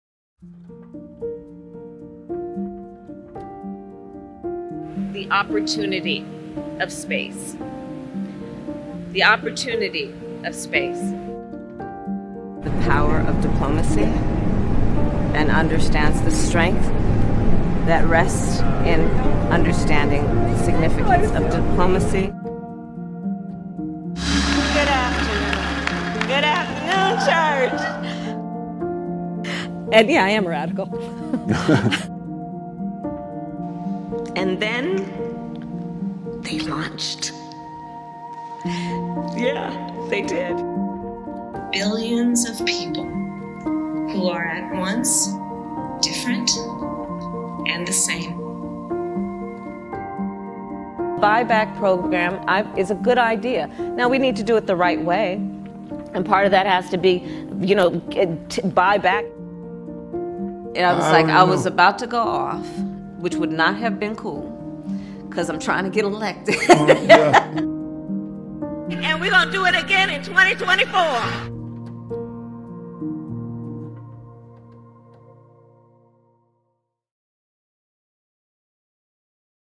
Veep Thoughts with Kamala Harris (Vol. 16)
Vice President Kamala Harris attempted to answer a question from a reporter on Thursday. Even though it was one of the easiest questions a politician has ever been asked—how do you feel about the good thing (Americans released from Russian prison) that just happened?—Harris absolutely flubbed the response.

"This is just an extraordinary testament to the importance of having a president who understands the power of diplomacy and understands the strength that rests in understanding the significance of diplomacy," Harris said in the style of a high school freshman asked to give a report on a book she hasn't read.